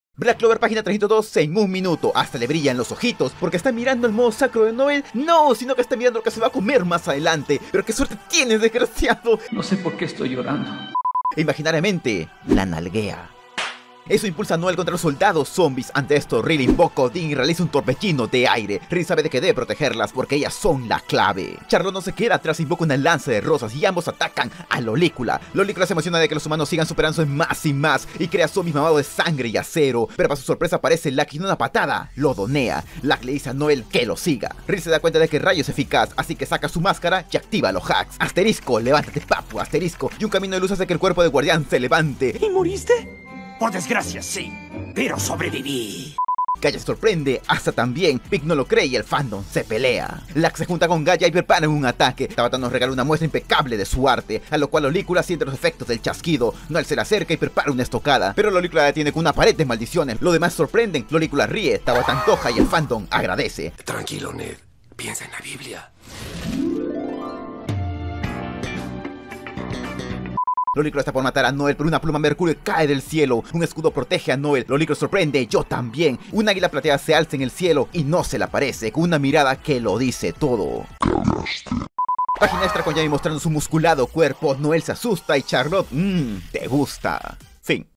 BLACK CLOVER 302 EN 1 MINUTO (más o menos) | RESUMEN MANGA
👋 Resumen del capítulo 302 de Black Clover donde Noelle con el poder del amor, esta lista para ponerle fin a l*licula pero el diablo es muy poderoso.  Ante esto Rill saca los hacks y alguien aparece...

Si te gusta One Piece y quieres un resumen que te saque una sonrisa, visita su canal.